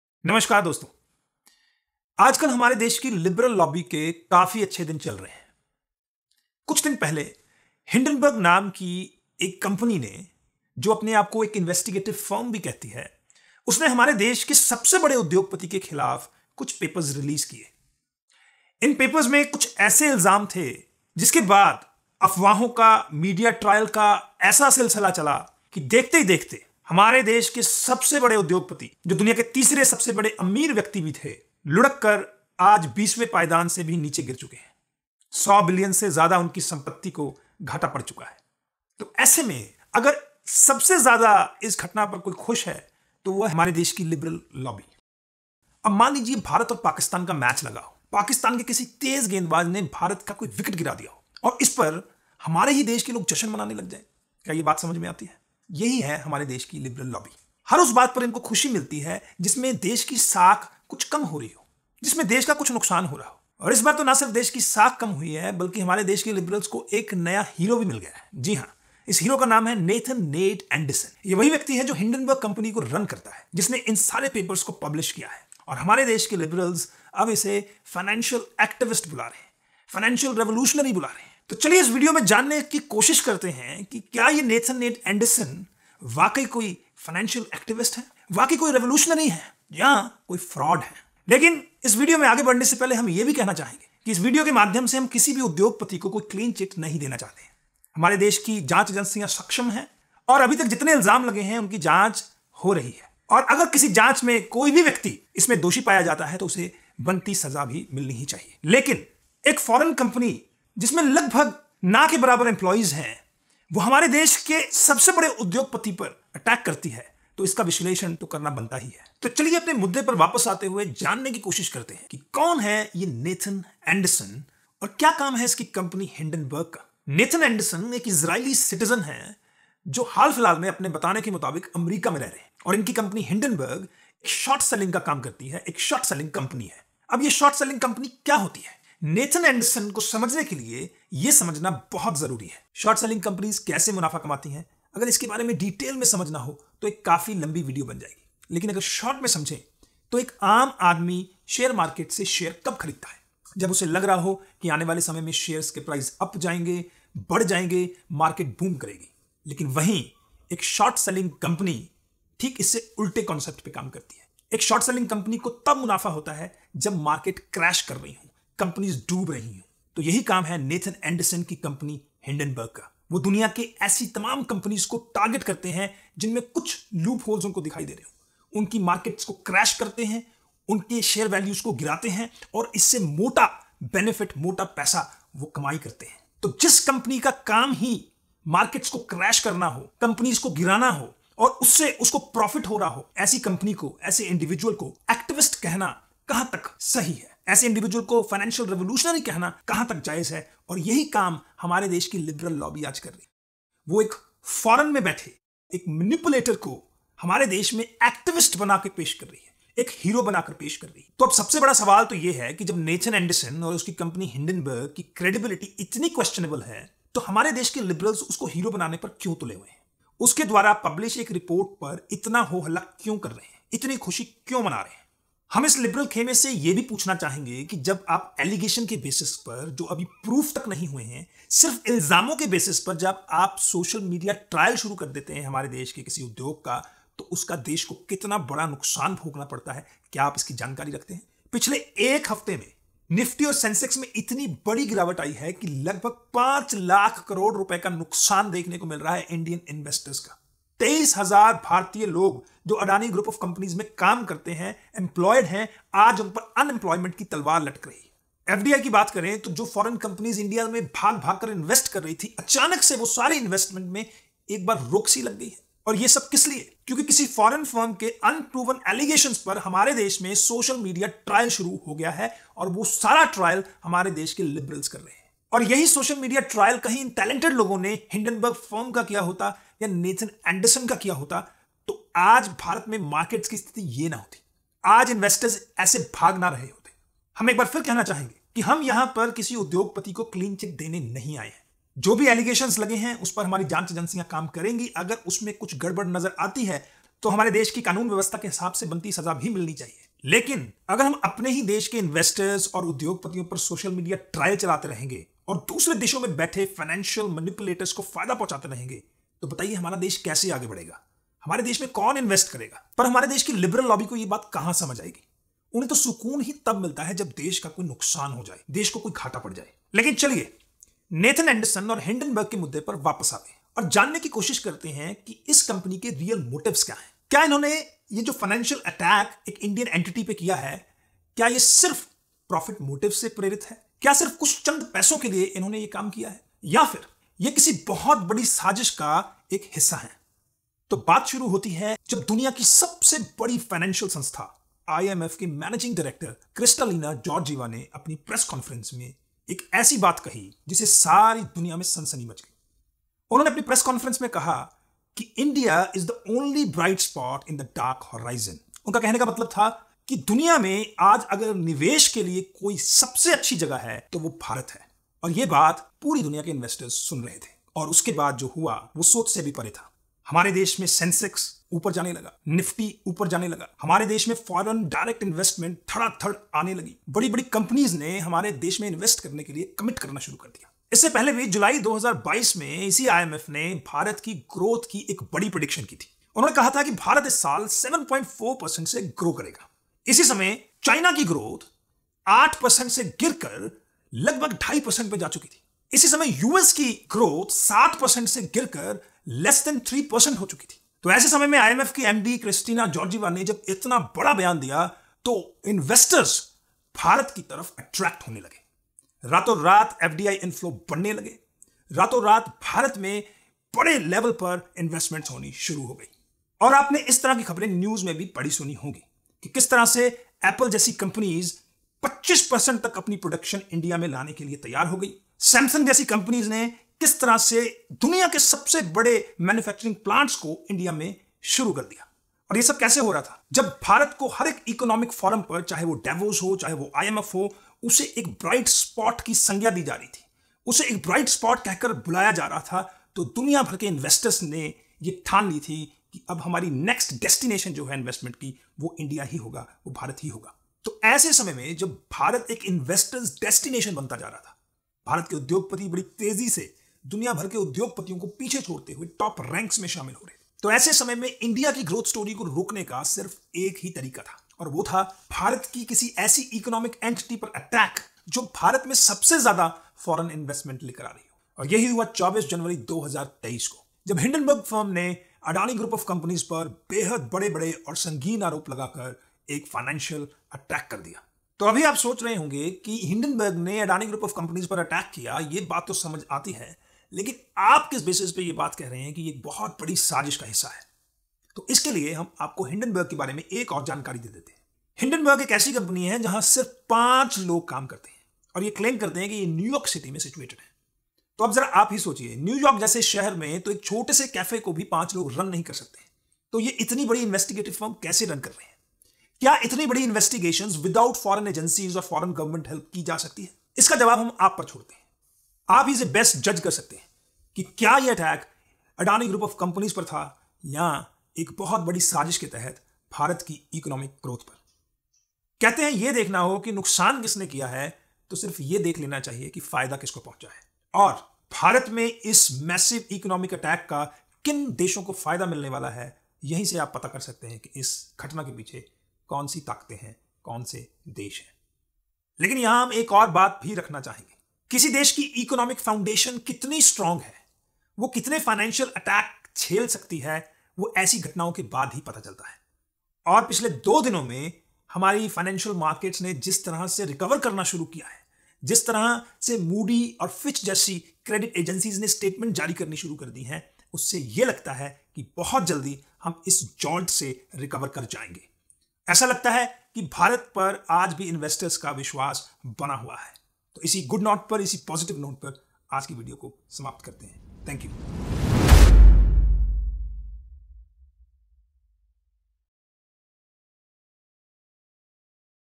Hindenburg Exposed | YPSS Video
This video while exposing the real motives of Nathan Anderson and his company Hindenburg behind publishing a controversial report against one of the most high ranking Indian Business Entity will also seek to address the question of who possibly is behind this Massive Financial Attack on India. People like Nathan Anderson are only pawns of the game: the real kings are always hard to catch.